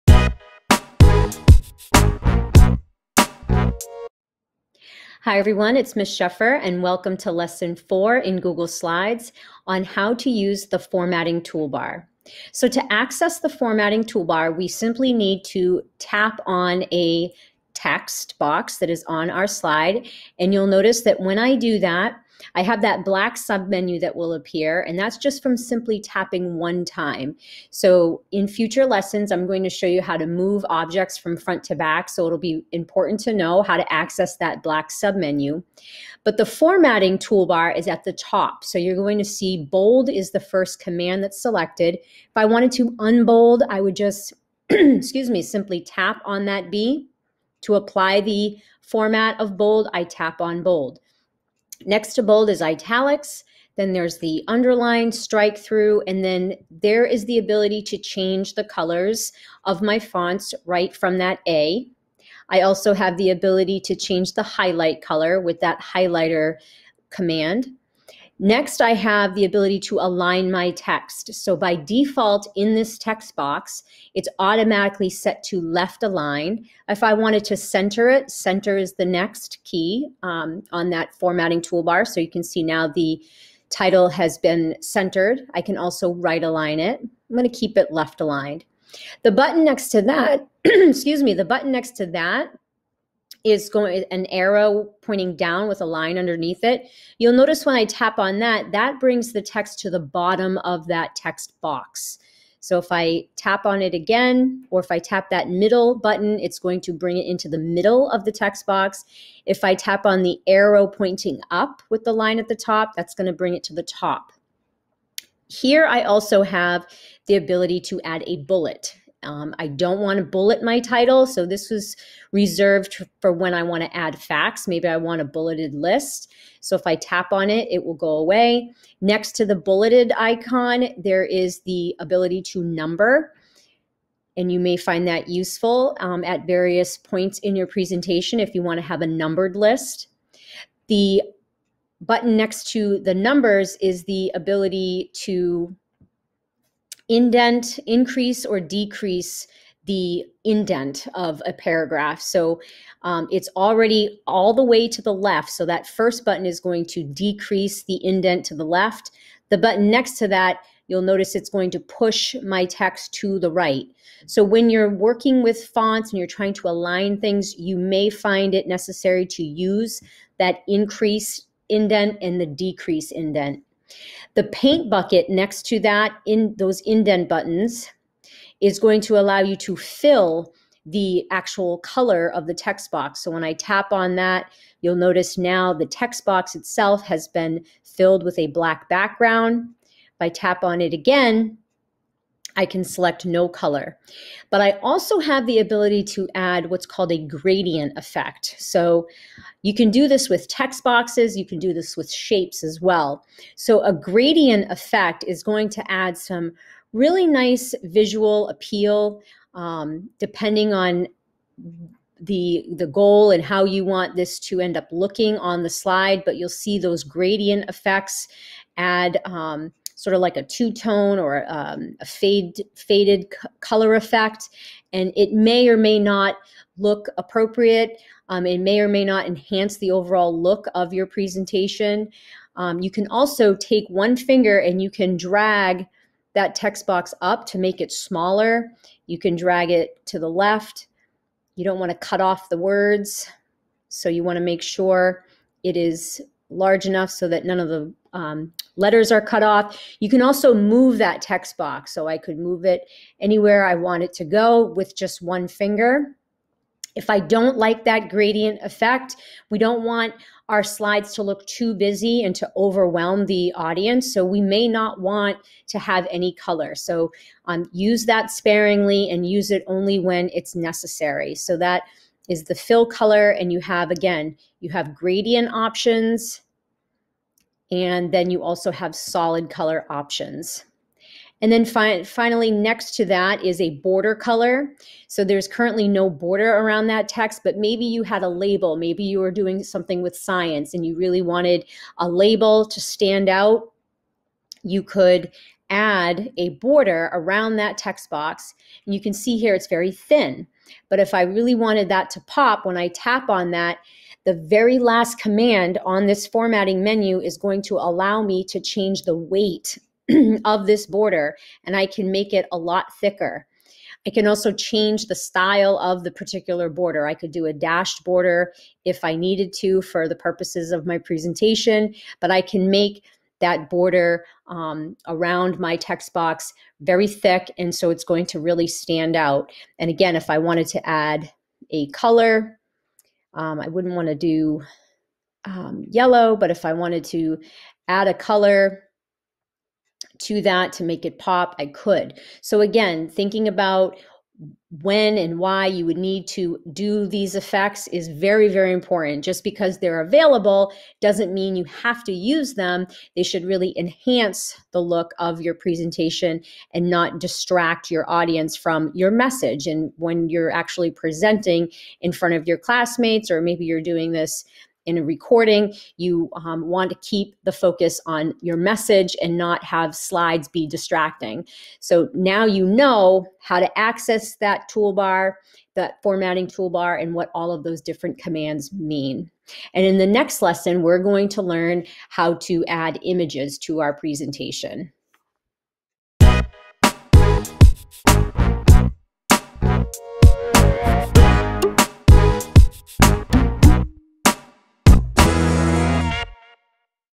Google Slides Lesson 4 Using the Formatting Toolbar
In this lesson, users will learn how to use the formatting toolbar.